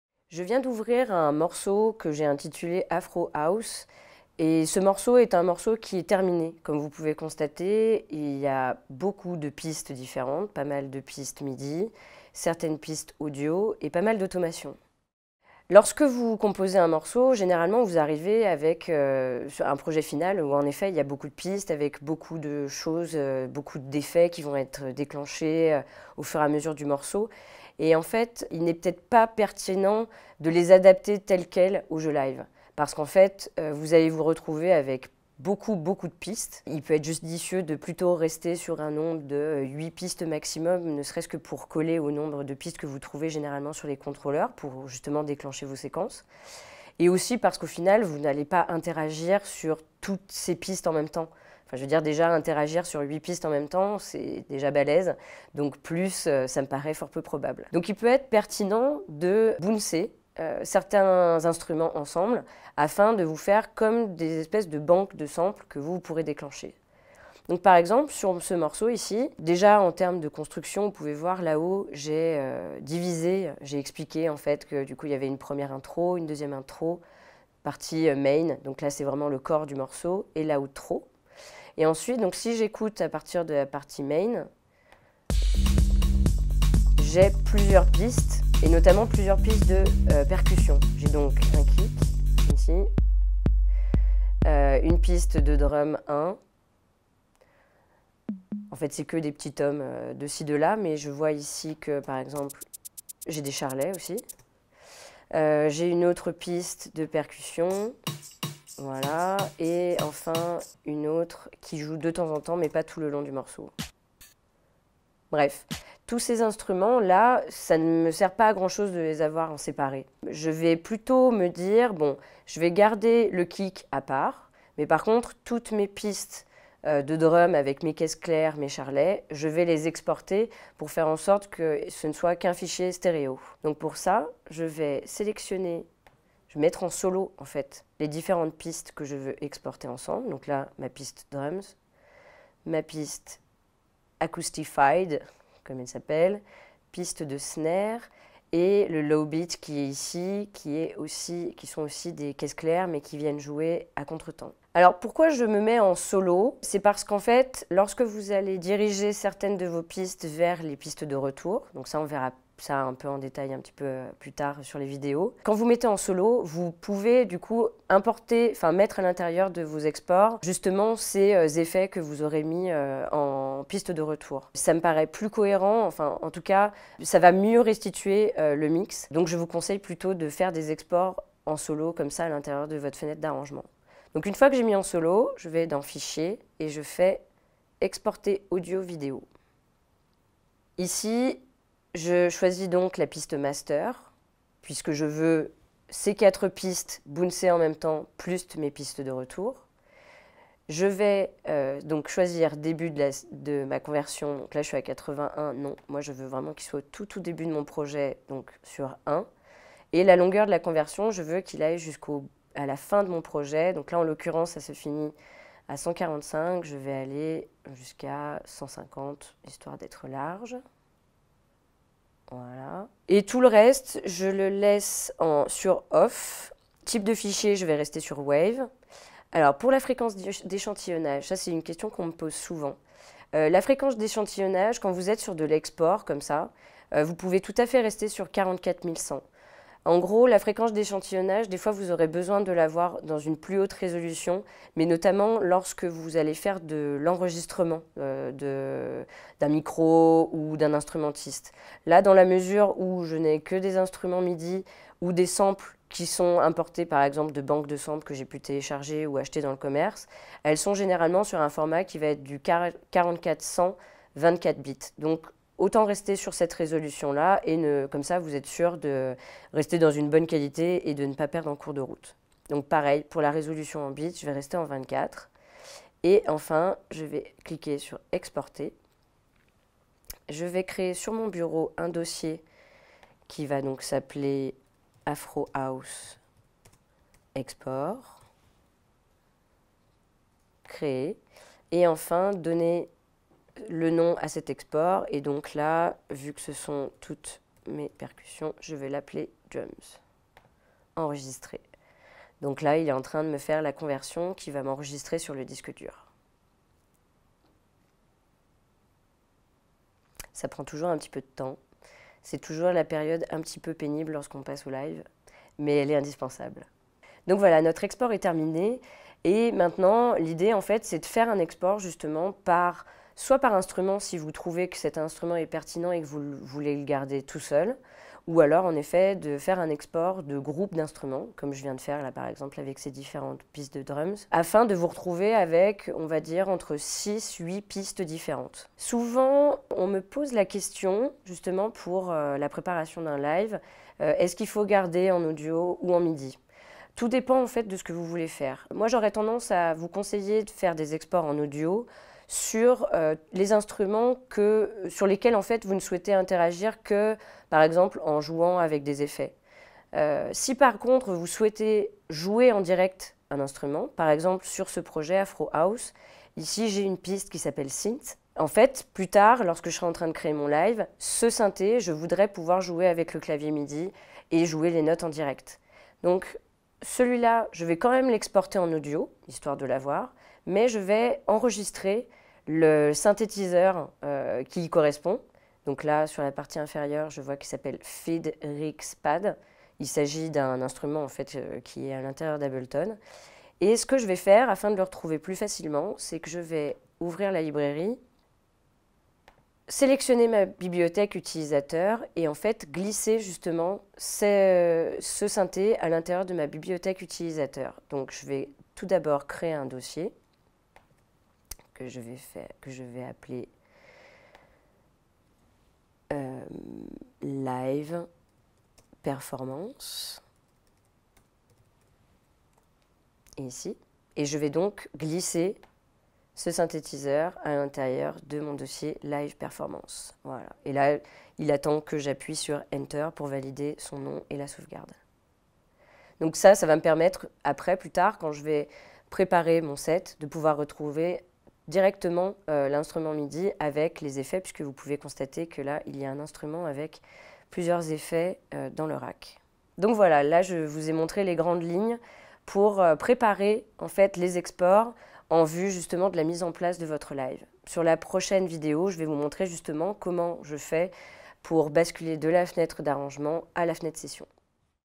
Tuto sur les outils MIDI dans Ableton 9
Dans cette ce tutoriel, vous apprendrez à grouper les pistes, exporter en audio et sauvegarder les outils MIDI dans Ableton 9.
Pour approfondir vos connaissances et compétences sur Ableton 9 pour la Performance Live, découvrez la formation complète